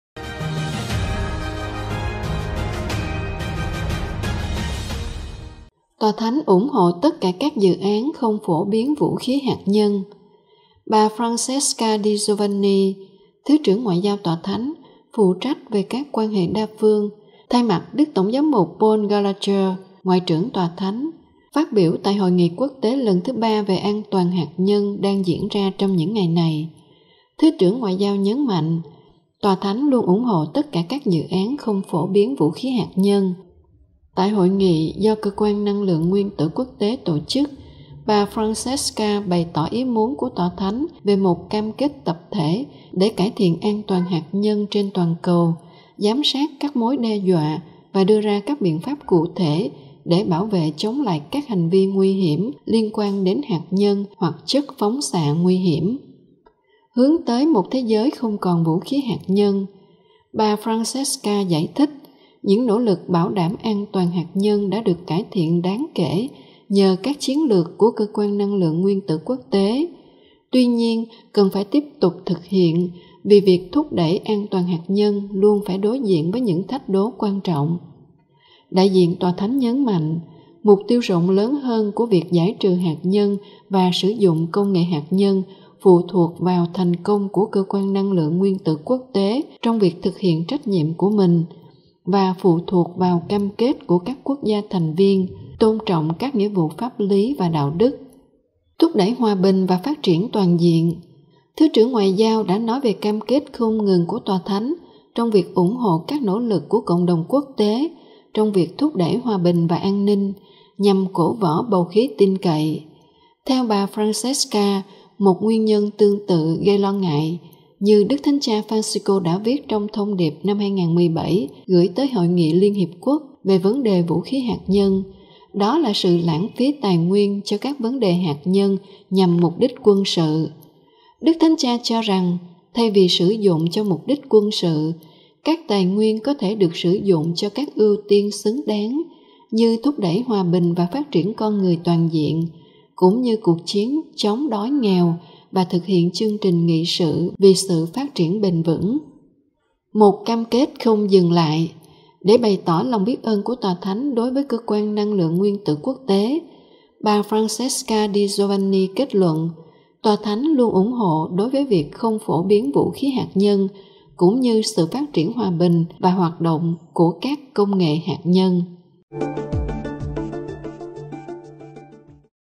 Tòa Thánh Ủng Hộ Tất Cả Các Dự Án Không Phổ Biến Vũ Khí Hạt Nhân
“Tòa Thánh luôn ủng hộ đối với việc không phổ biến vũ khí hạt nhân, cũng như sự phát triển hòa bình và hoạt động của các công nghệ hạt nhân”.